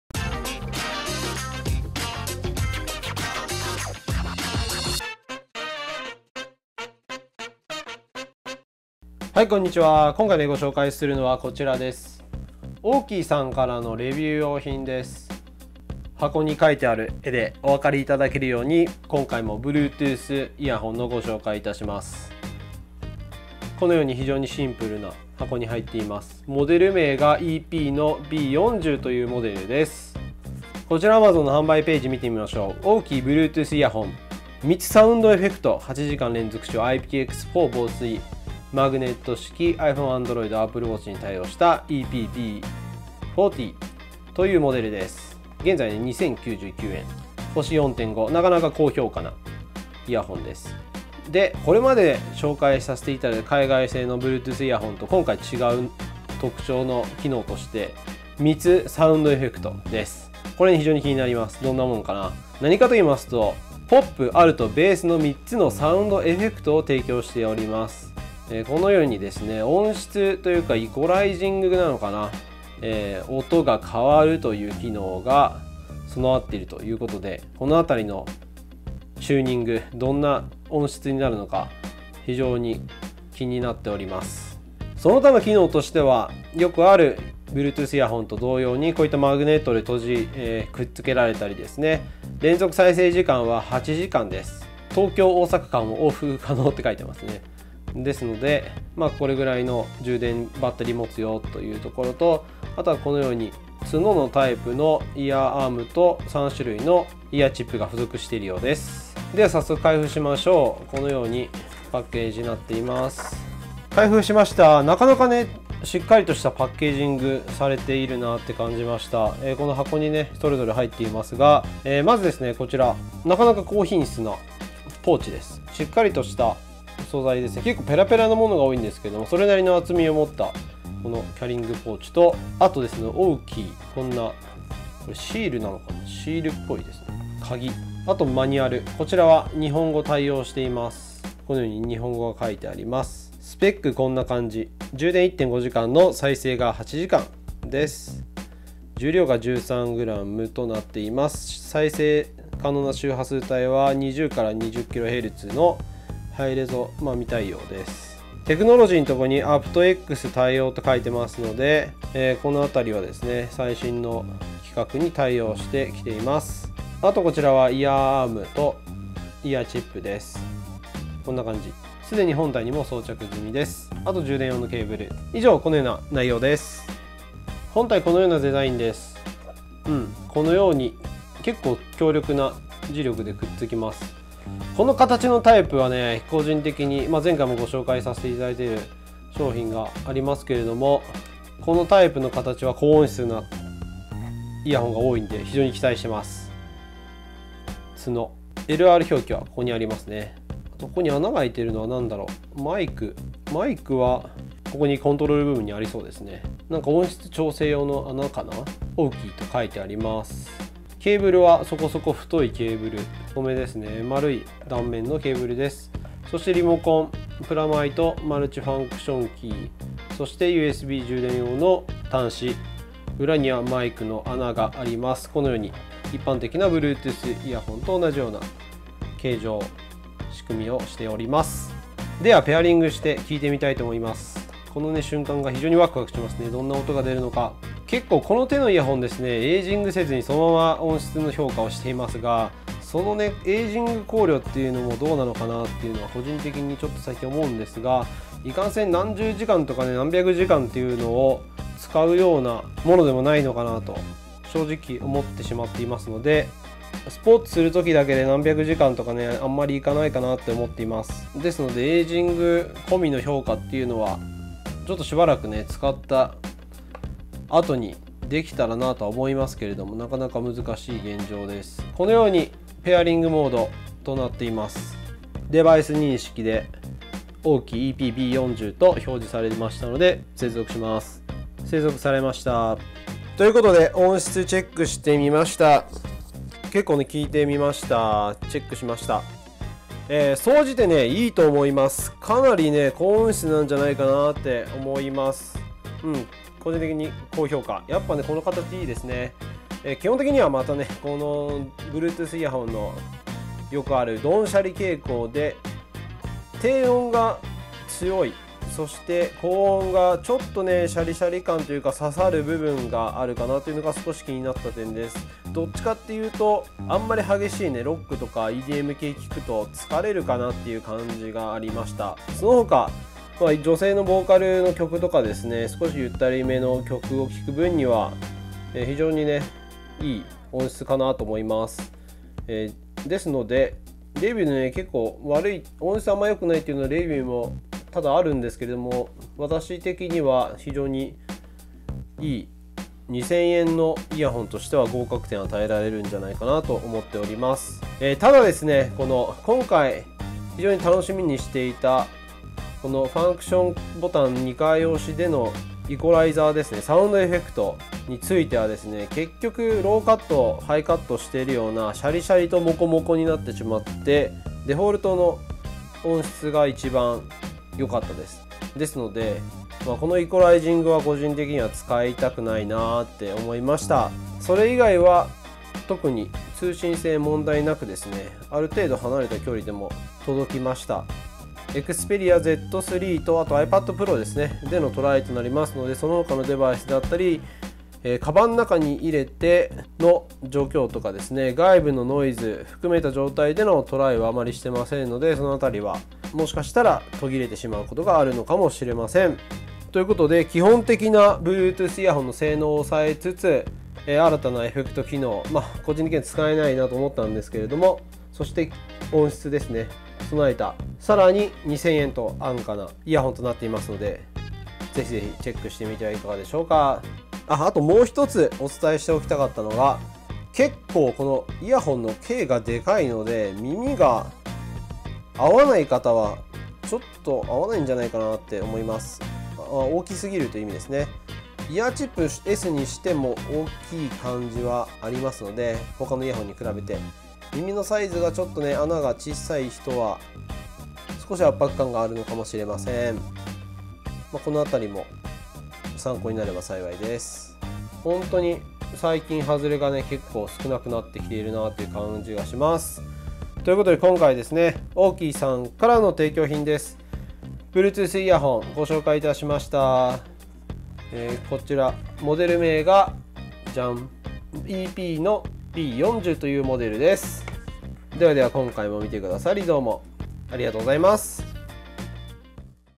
サウンド・エフェクト付！？AUKEY bluetoothイヤホン EP-B40
ベースの音は良いのですが、サウンドエフェクト機能は不要かな。

AUKEY bluetoothイヤホン 三つサウンドエフェクト 8時間連続使用 IPX4防水 マグネット式 iPhone、Android、Apple Watchに対応 EP-B40
※レビュー用提供品です